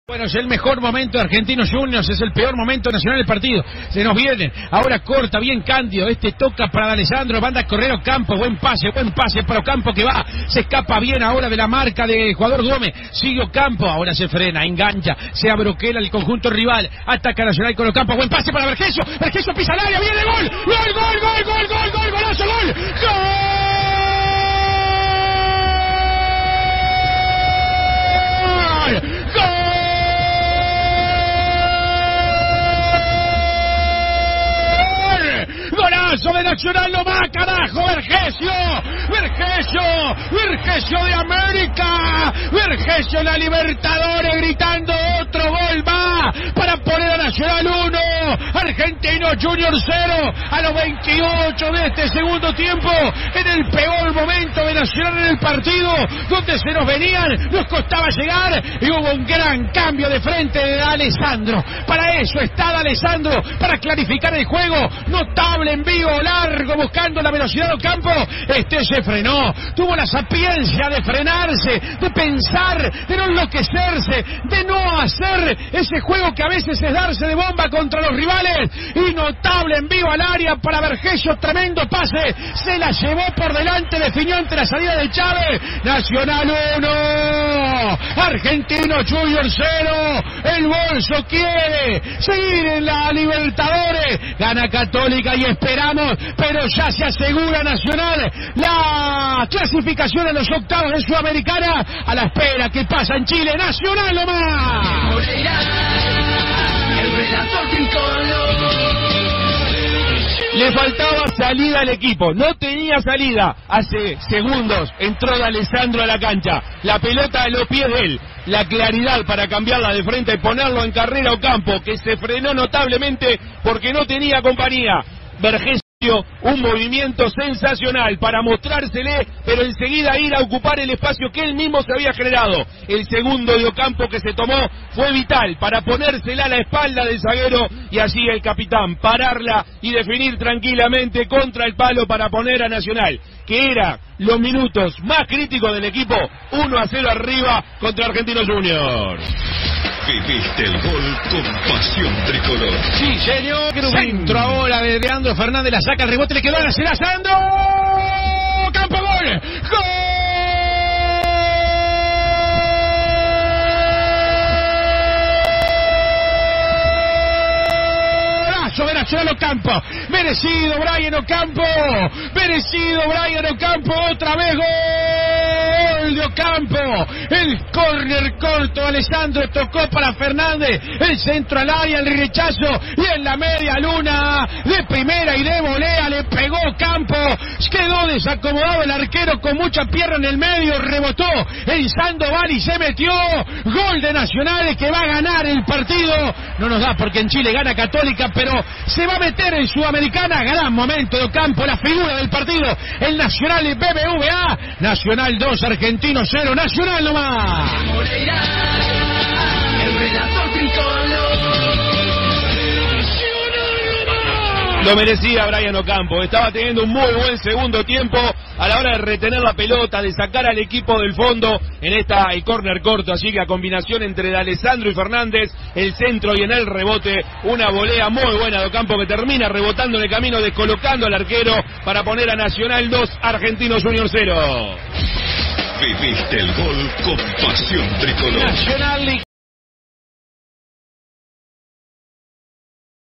Nacional 2 Argentinos Juniors 0 - Libertaodres 2021 (por Javier Moreira)
Los goles del partido, por Javier Moreira para Pasión Tricolor, de CX24 1010 AM.